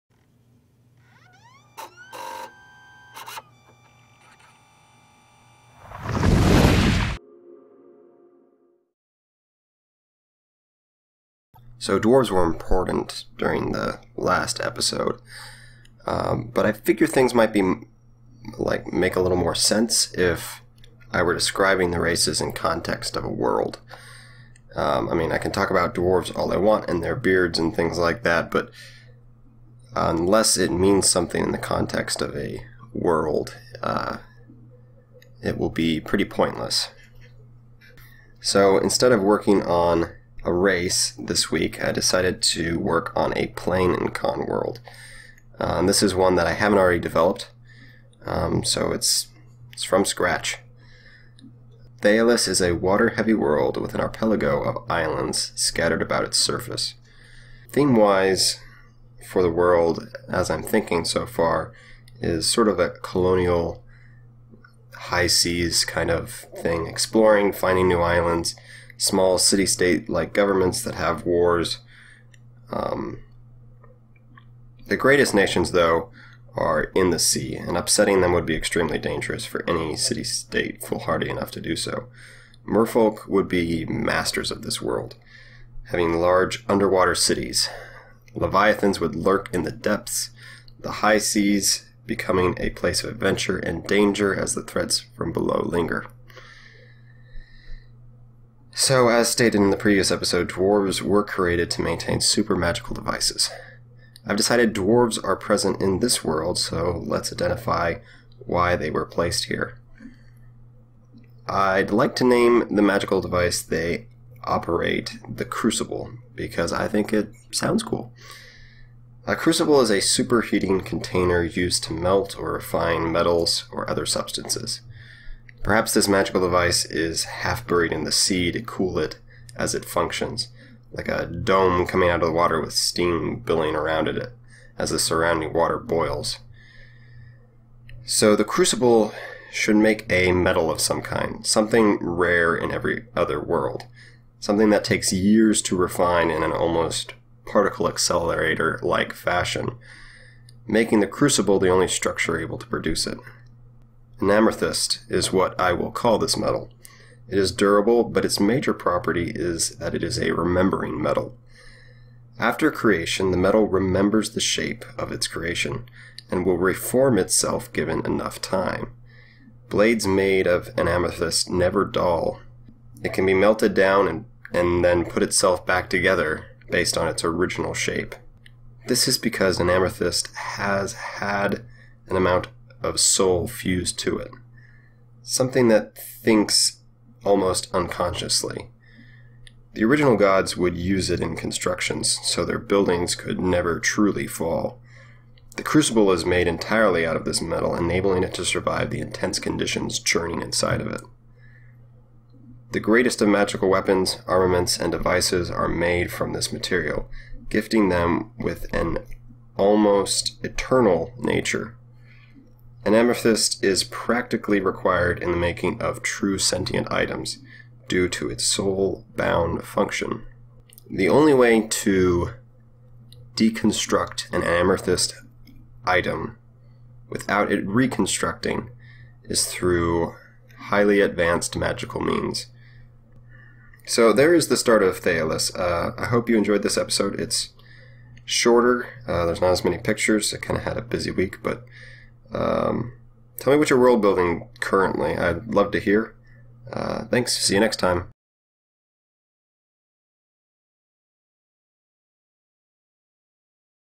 CONWOLRD -004- A Start To Thayaliss
Back into CONWORLD my D&D worldbuilding series. If you listen carefully you can hear my asthma!!!